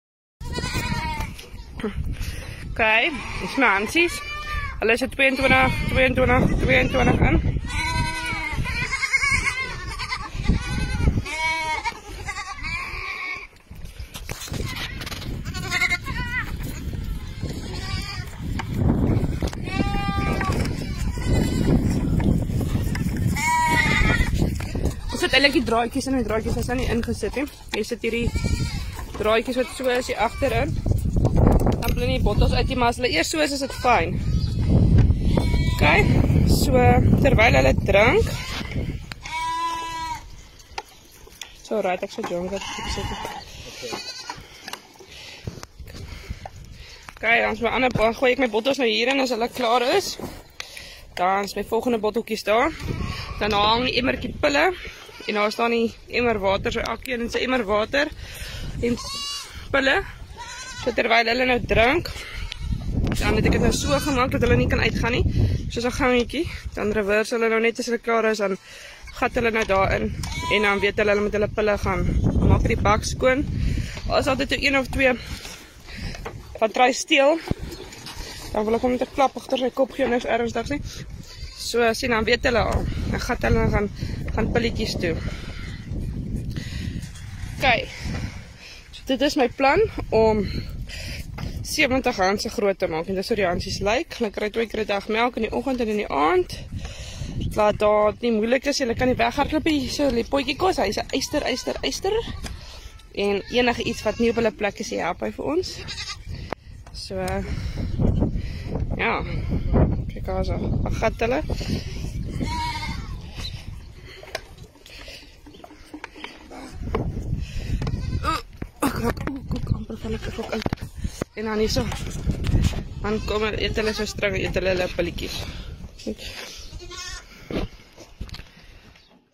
Voer Baie Hanslammers gelyk
Kyk die slim plan om meer as een hansie gelyktydig te voer